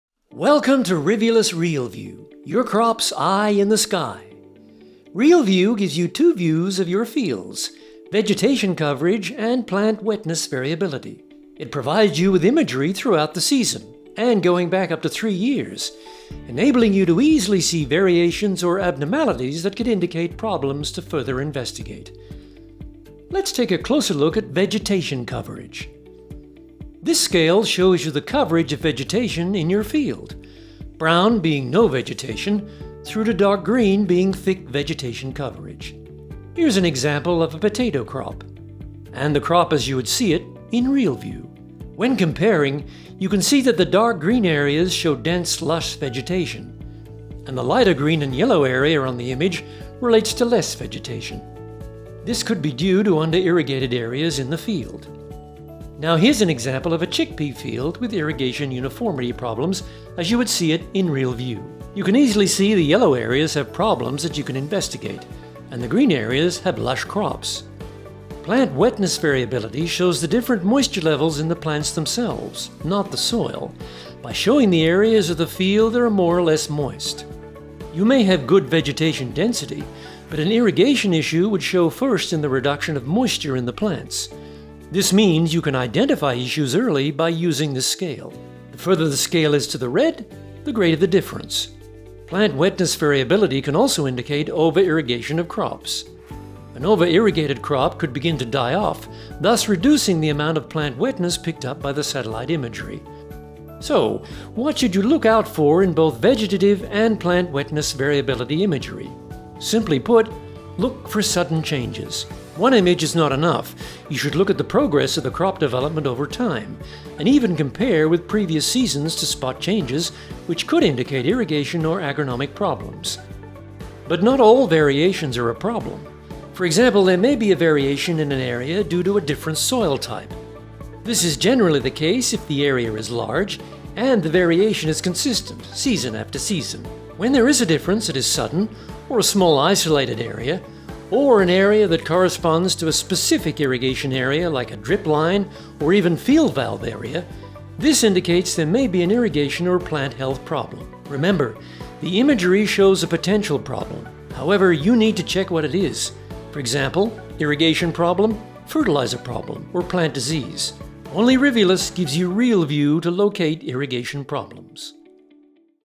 Rivulis ReelView Quick Guide: How to read field imagery
ReelView, your crop’s eye in the sky.
Agronomic satellite imagery of your crops from your phone. FREE with every roll of Rivulis drip line/tape*